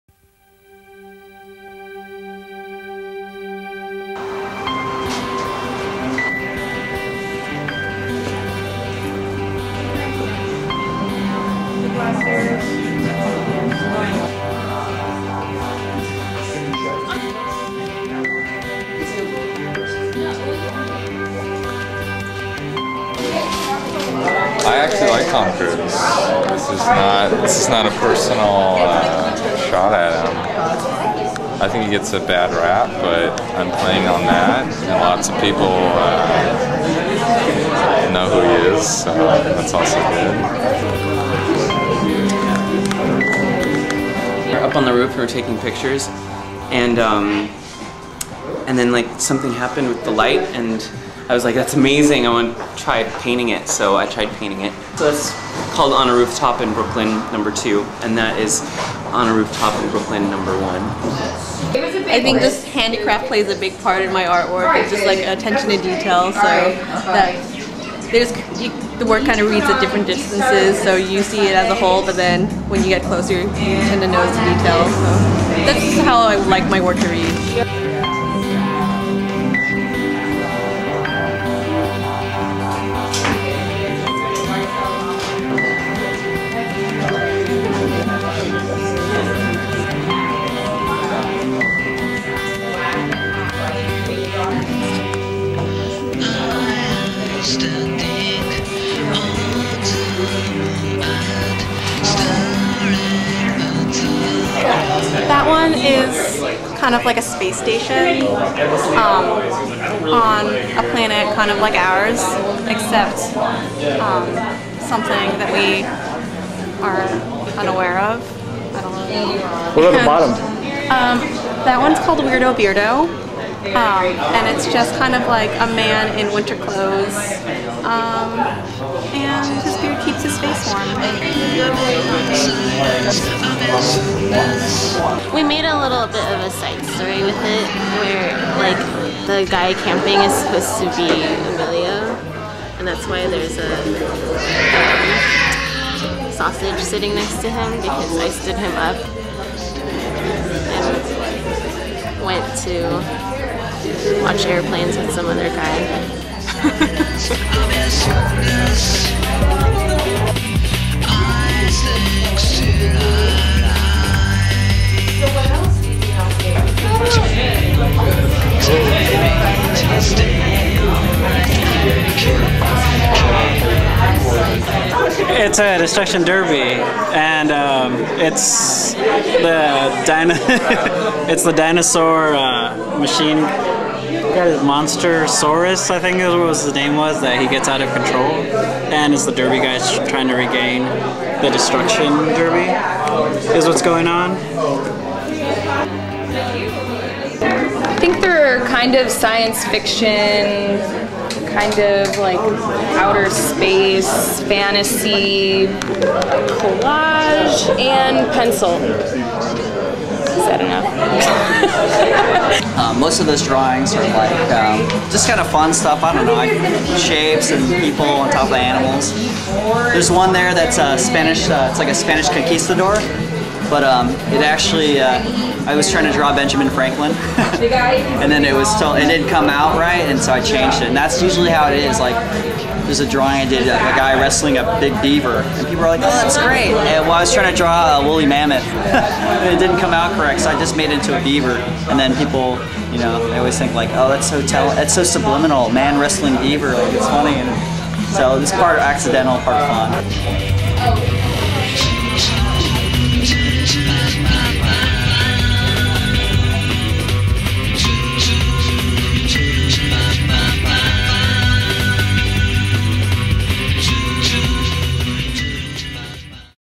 Giant Robot 2 - Suggestion Box
Reception: Saturday, July 19, 6:30 -10:00

Giant Robot is proud to present Suggestion Box, a group show featuring new works by artists selected by our own store and magazine staffs in addition to pieces by many of the staffers themselves.

We intend our publication and shops to be catalysts as well as showcases, and are excited to mix artists that provide inspiration with homegrown talent that is inspired by them. Pieces will include drawings, paintings, comics, sewn work, and other mediums.

Contributors will include (but are not limited to) the following:

Aaron Brown
Jeffrey Brown
Seth Drenner
Jordan Fu
Michael Hsiung
Benjamin King
Diana Kwok
David Magdaleno
Tru Nguyen
Molly Colleen O'Connell
Sidney Pink
Yumi Sakugawa
Emilio Santoyo
Eric Shaw
Ryan Jacob Smith
Mark Todd
Christiaan Van Bremen
Katie Vonderheide